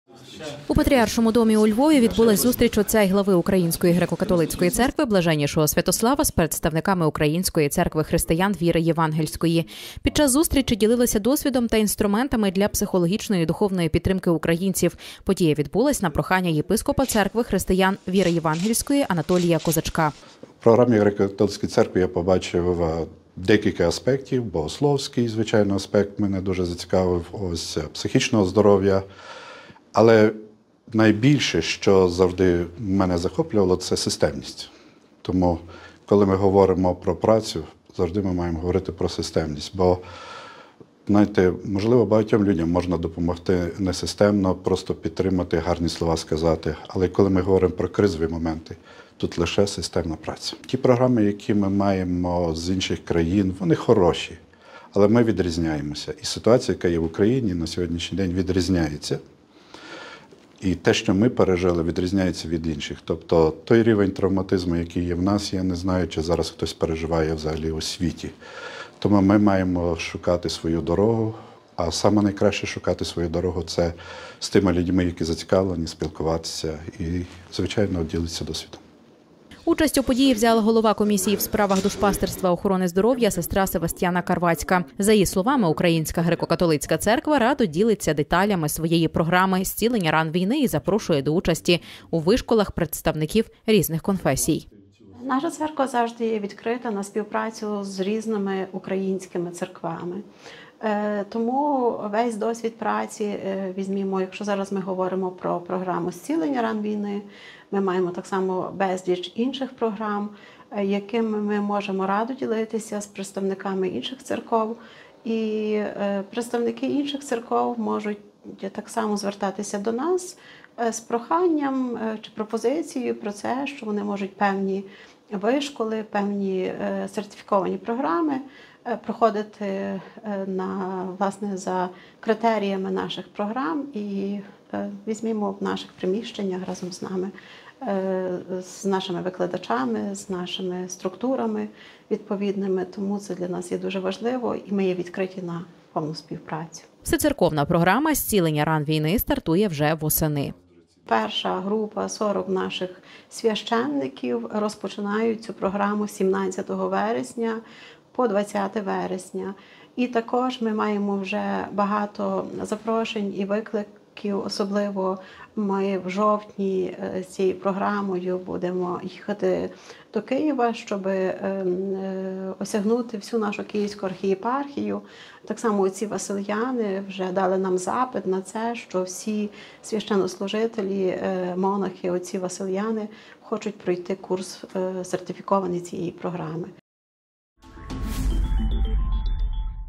Зустріч Отця і Глави УГКЦ із представниками Української Церкви християн віри Євангельської
У Патріаршому Домі у Львові відбулась зустріч Отця і Глави УГКЦ Блаженнішого Святослава із представниками Української Церкви християн віри Євангельської. Під час зустрічі ділились досвідом та інструментами для психологічної і духовної підтримки українців.

Поширюйте відео!

Україна молиться!